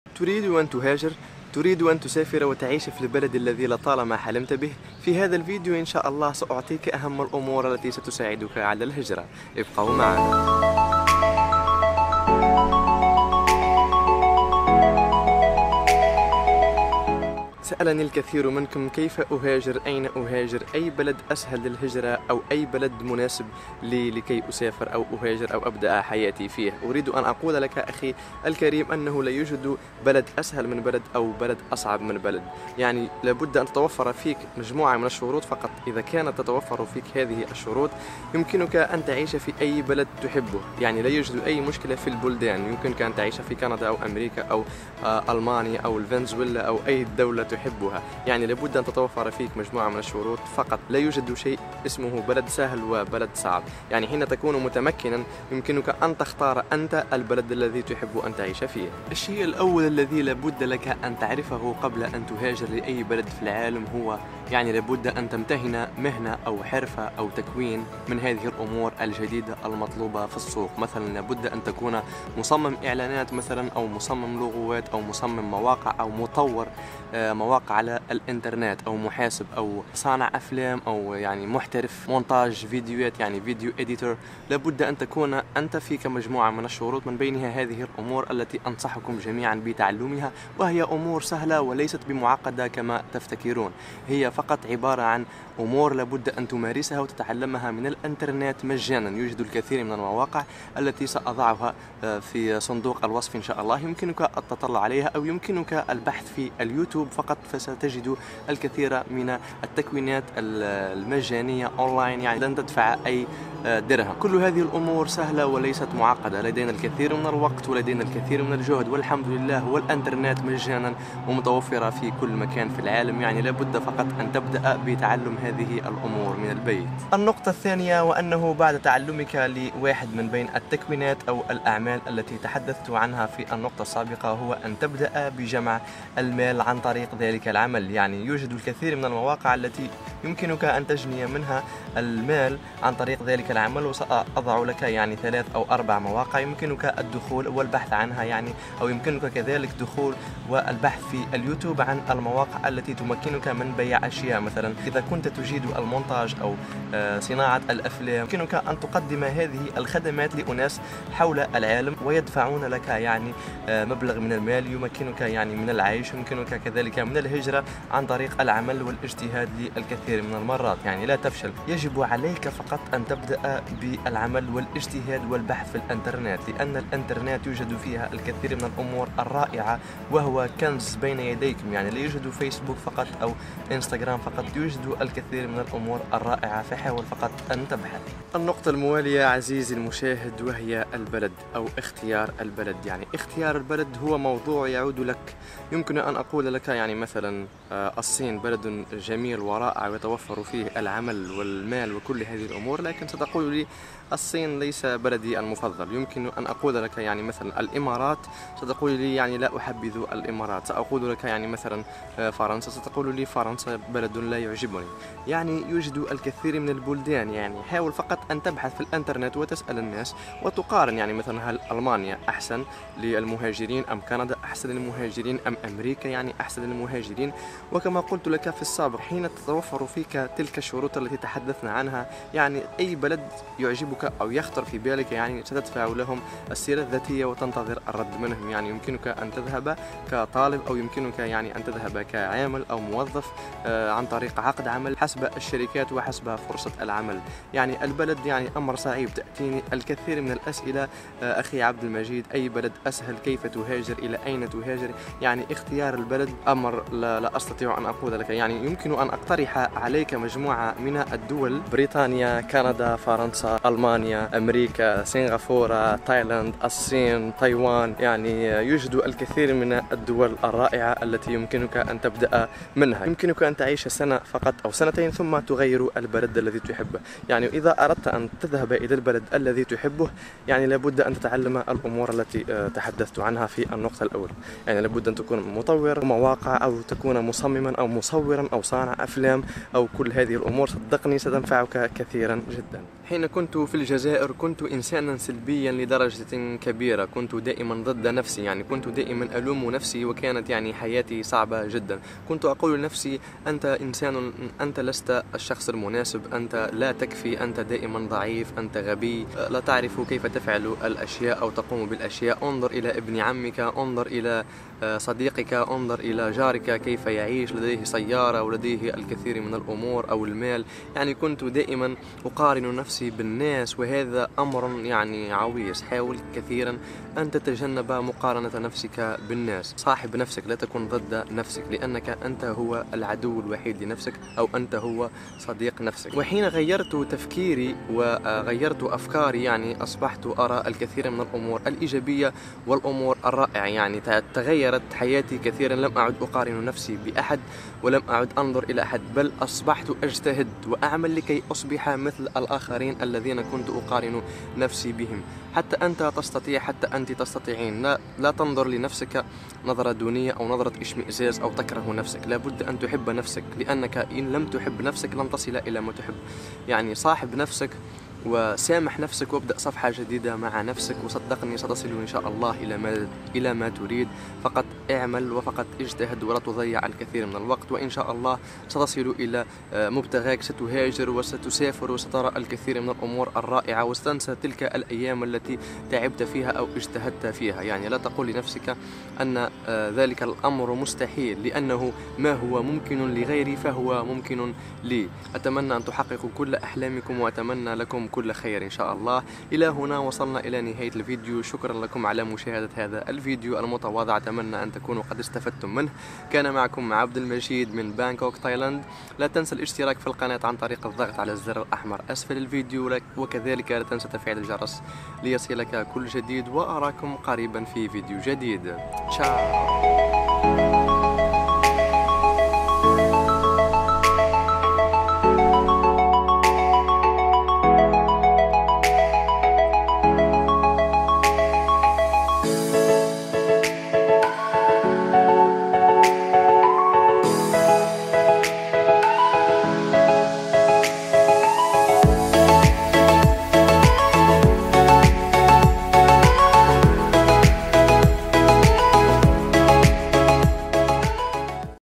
أحسن طريقة للهجرة 2020
في هذا الفيديو سأوضح لكم أحسن طريقة للهجرة وأهم الأمور التي يجب أن تتبعها لكي تهاجر الى البلد الذي تحلم بالعيش فيه

المواقع التي يمكنك التعلم منها ببلاش
المواقع التي يمكنك جني المال منها

معدات التصوير | My gear

with any questions